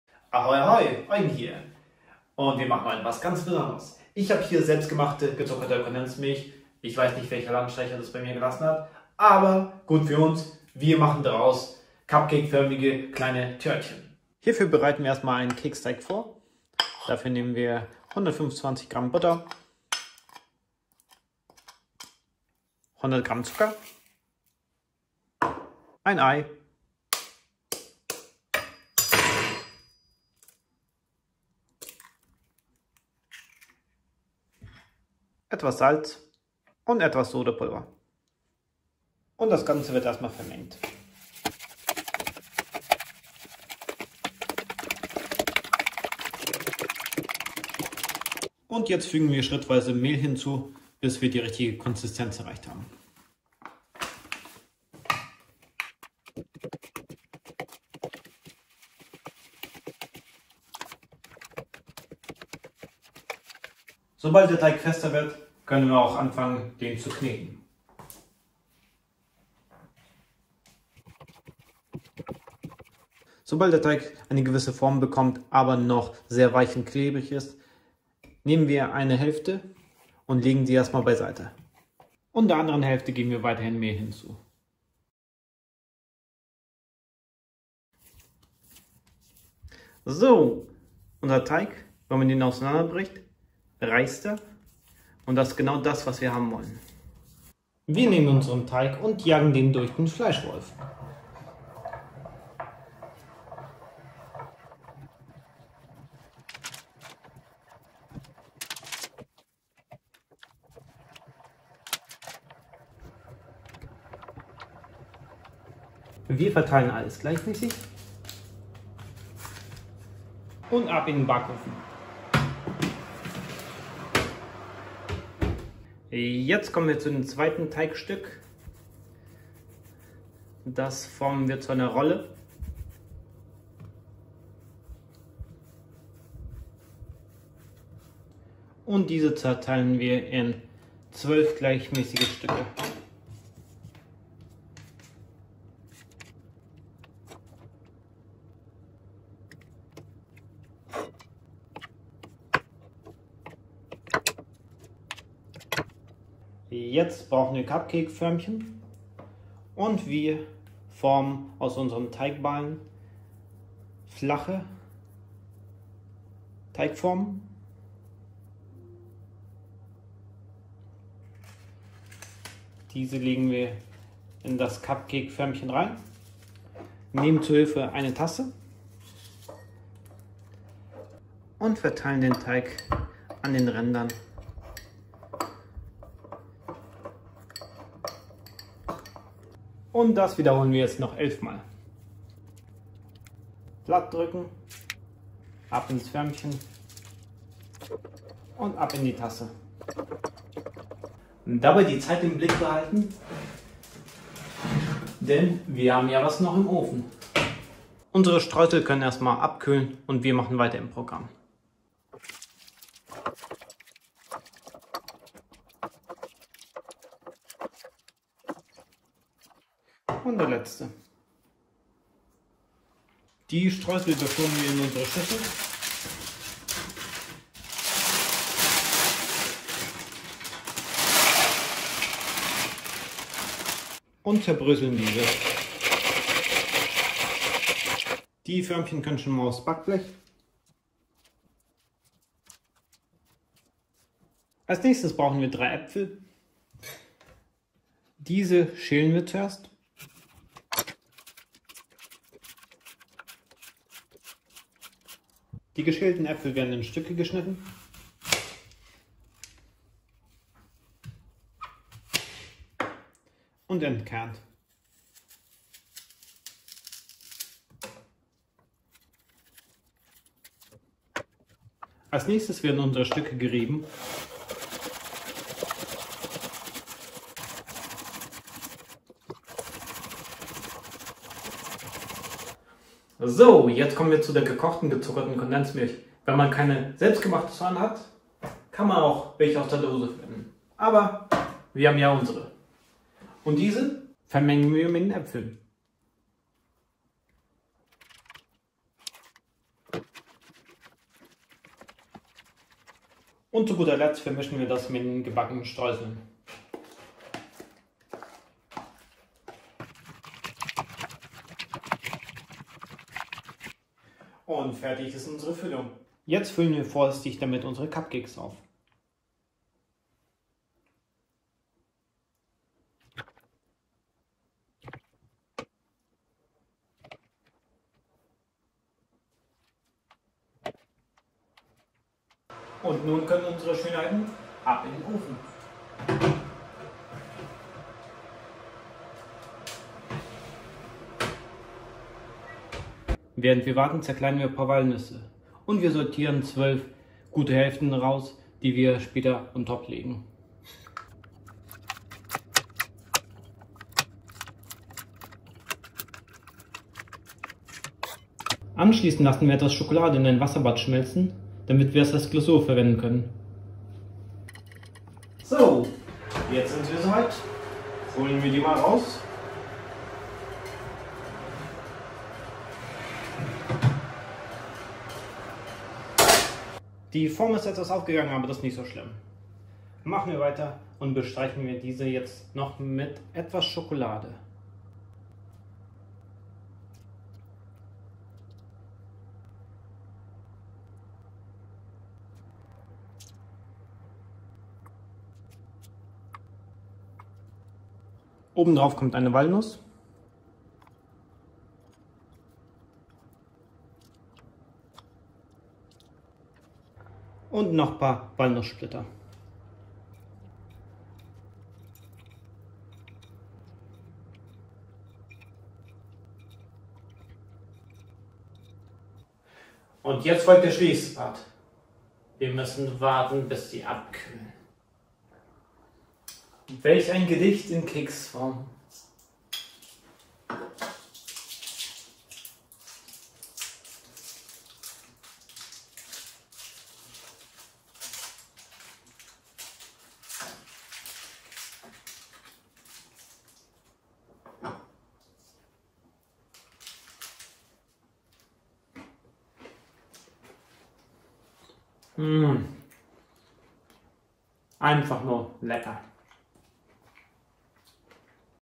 DIY Kochstudio: Cupcakes - Staffel 2: Episode 6
Wenn man grad gekochte gezuckerte Kondensmilch zur Hand hat,
ist die einzig logische Konsequenz, dass...
man leckere Cupcakes damit macht!!!